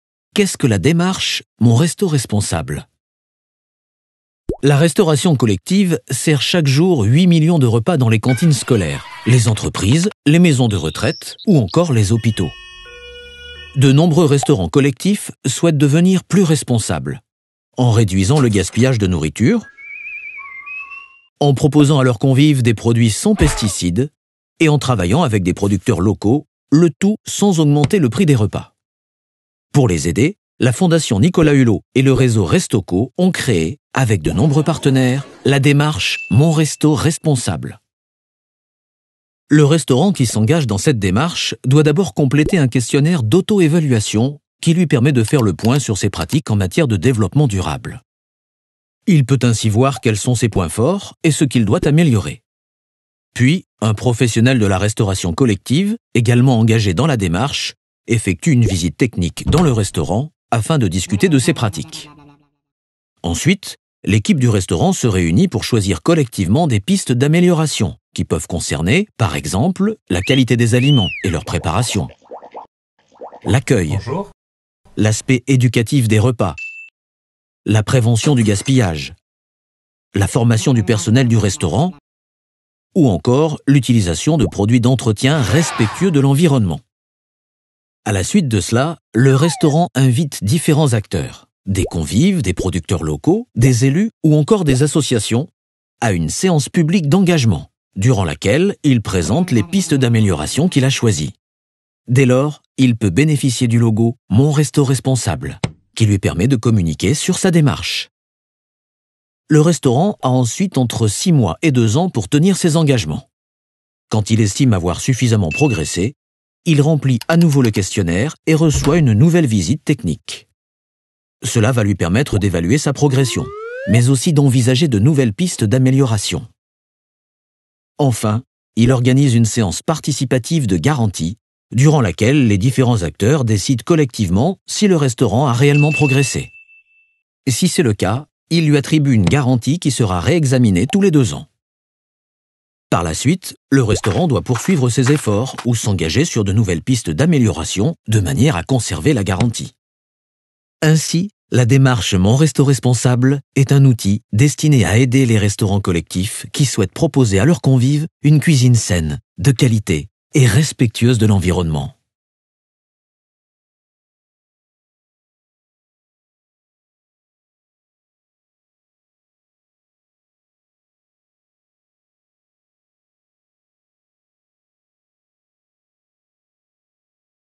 © Fondation pour la Nature et l'Homme - Mon Restau Responsable®
Mercredi 19 octobre 2022 , le restaurant des Lacs de l’ESAT Métropole, géré par l’Adapei de la Gironde, rejoint la garantie participative Mon Restau Responsable®, créée par la Fondation pour la Nature et l’Homme et le réseau Restau ’Co.